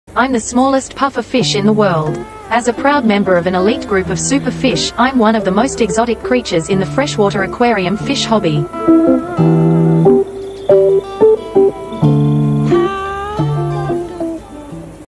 I'm the smallest Puffer Fish in the world!
I'm the smallest Puffer Fish in the world! As a proud member of an elite group of super-fish, I'm one of the most exotic creatures in the freshwater aquarium fish hobby.
MICRO AQUATIC SHOP
Unit 15, 2-8 Daniel Street, Wetherill Park, NSW 2164